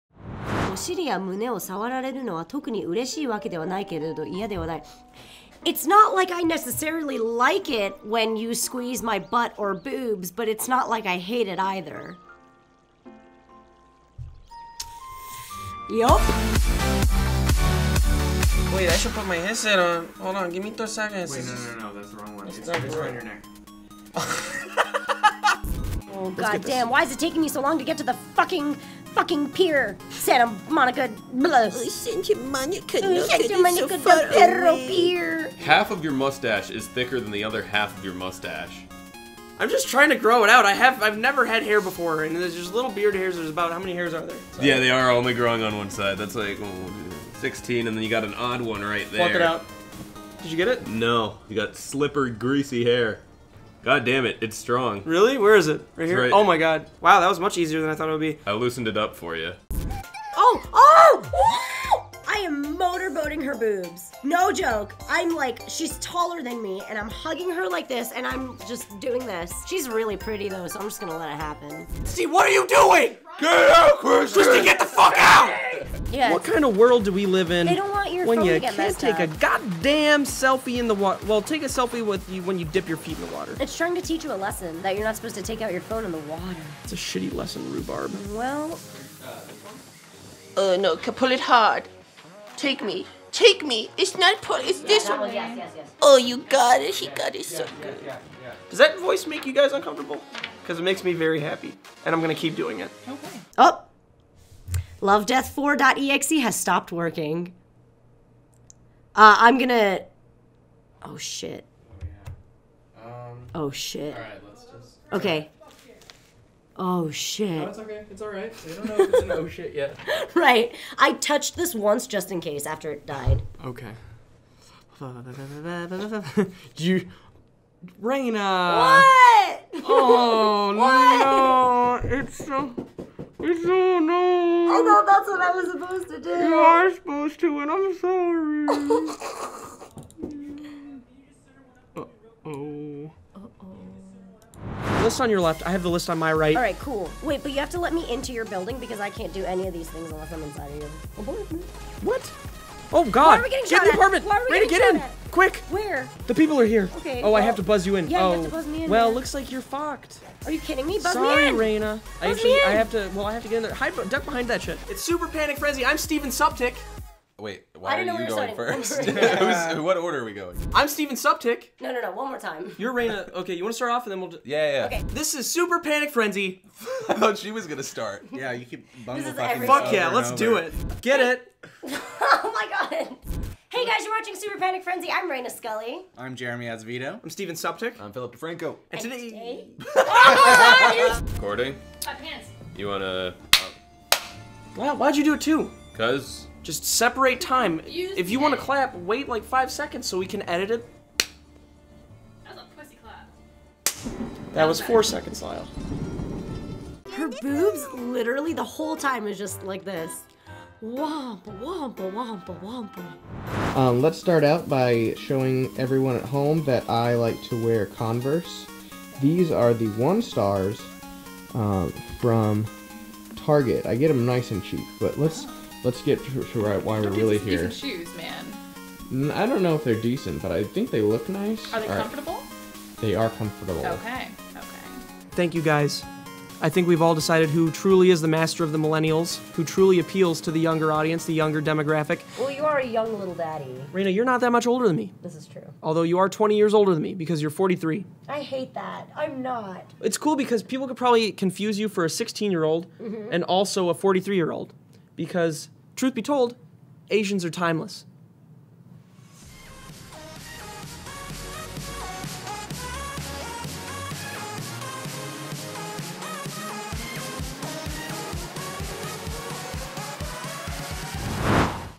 ASIANS ARE TIMELESS (SuperPanicFrenzy BLOOPERS #2)
Common known fact: Asians are neither born nor can they die. Reina is immortal. Her power is endless.

Hosts: @mlghwnt @ReinaScully @PhillyD

Guests: @LyleMcDouchebag @Dangersharkz @kezfx @MattEatsMochi

Edited by: @kezfx

ATTN: SuperPanicFrenzy
6433 Topanga Canyon Blvd #805